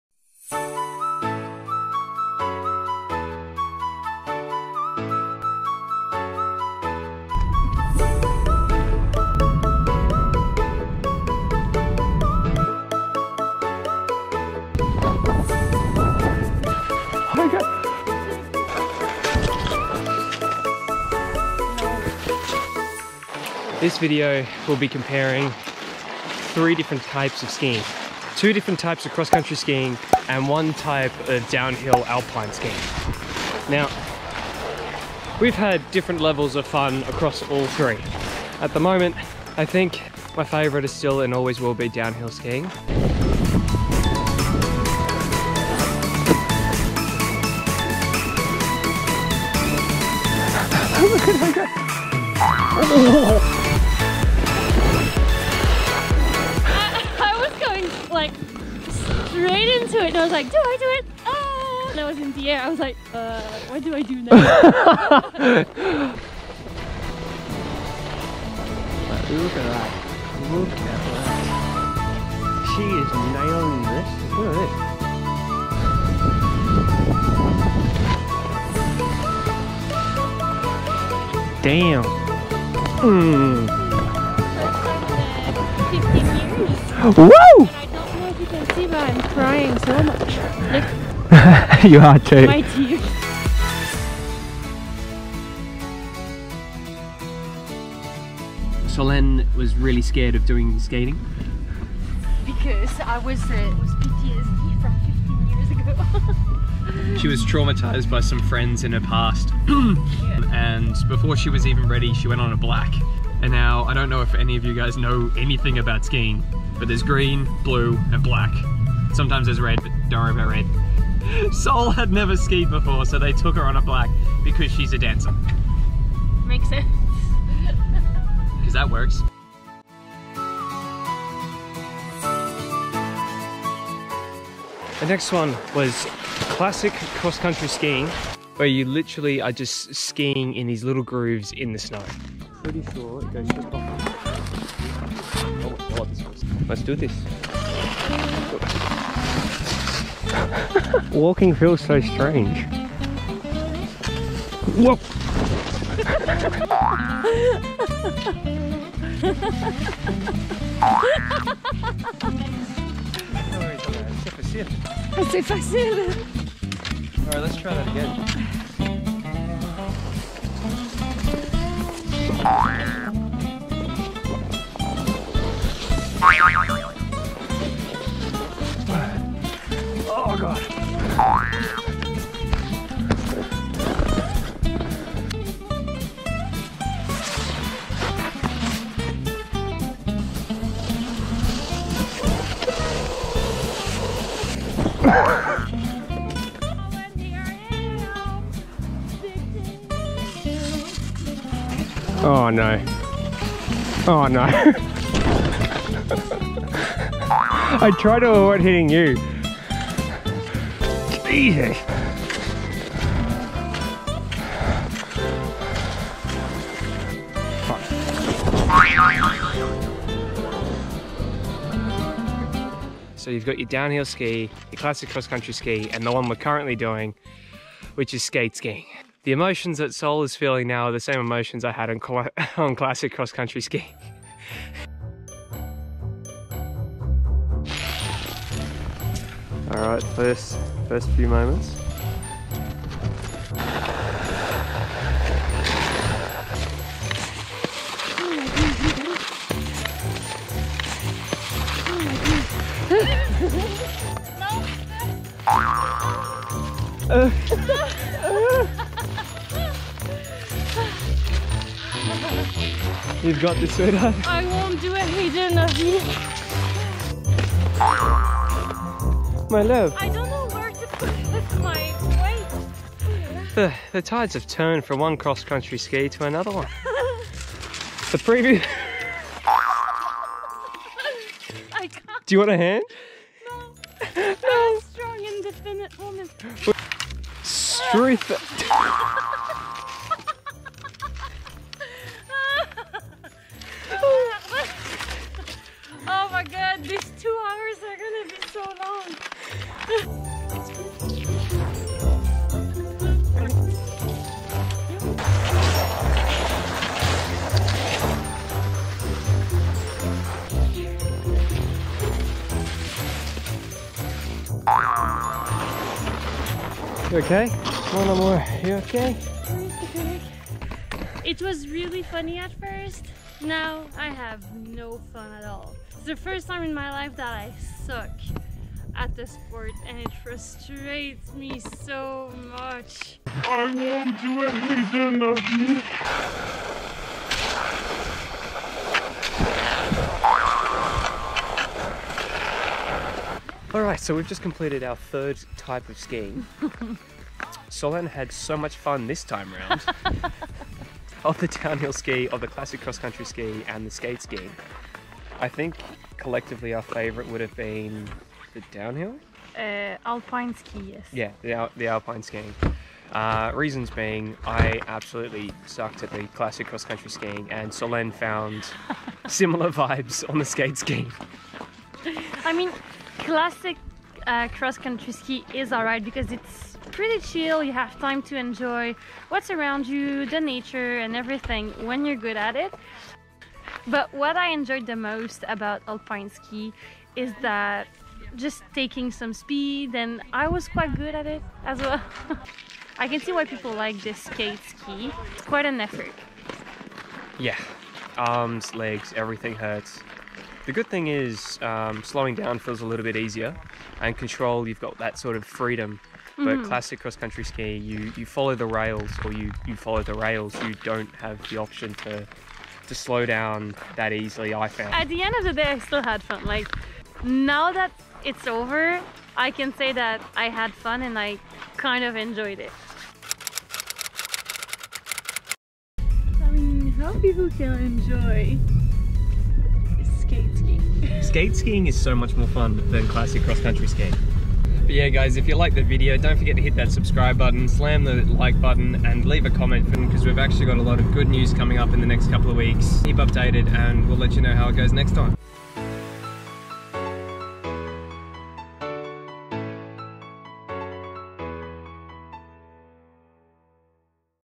CROSS COUNTRY SKIING SUCKS (Our Honest Opinions) - Episode 7
Seriously, it hurt our pride more than falling over that many times!

Places we skied:
- Foret Recreative for the cross country variants

Gear:
DJI Mavic Pro - t.ly/pMha
GoPro Hero8 - t.ly/9TJy
Canon T5i - t.ly/Ynu3
18-135mm Lens - t.ly/t8Bu